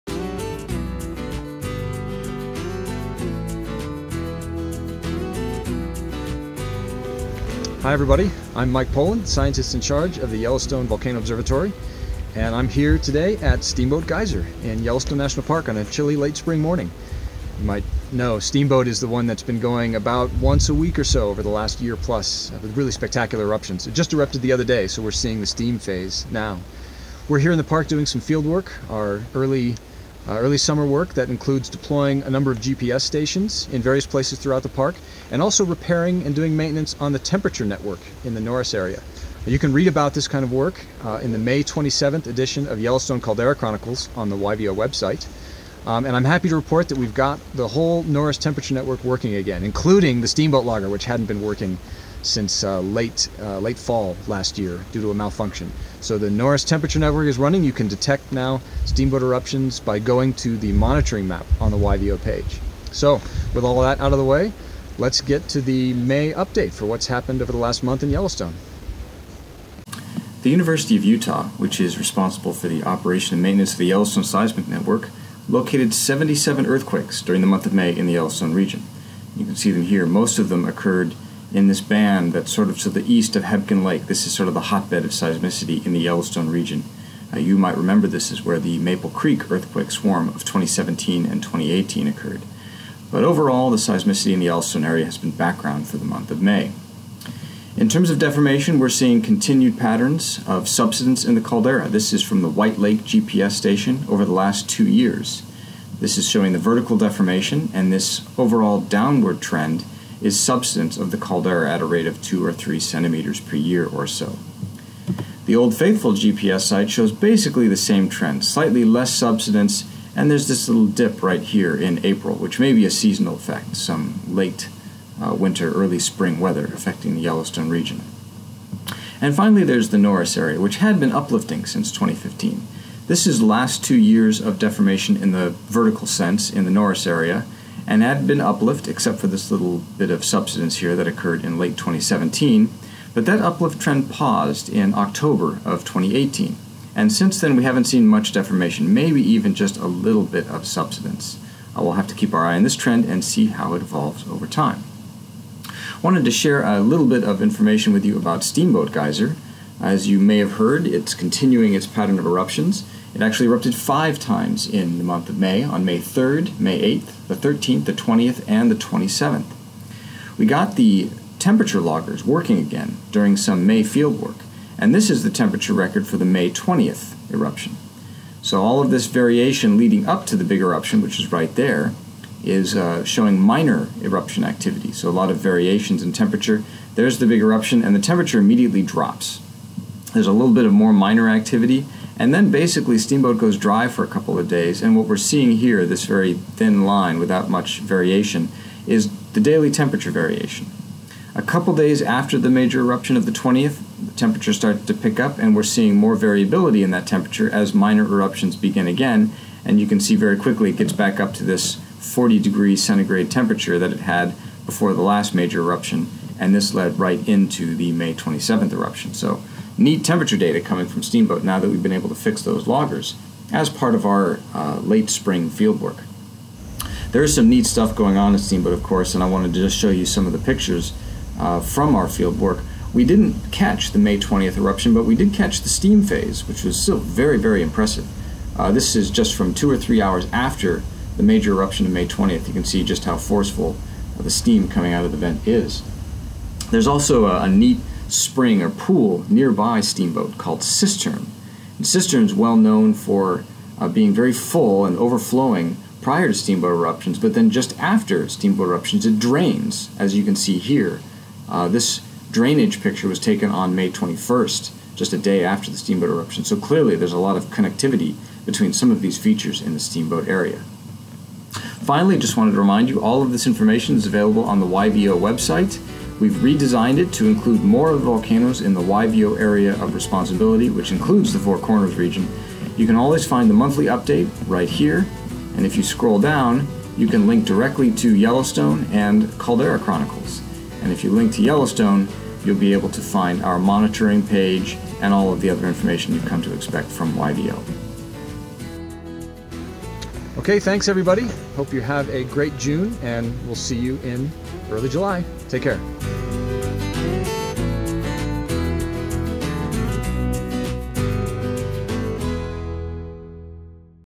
Yellowstone Volcano Observatory Monthly Update: June 3, 2019
Mike Poland, Scientist-in-Charge of the Yellowstone Volcano Observatory, describes activity at Yellowstone during the month of May, 2019.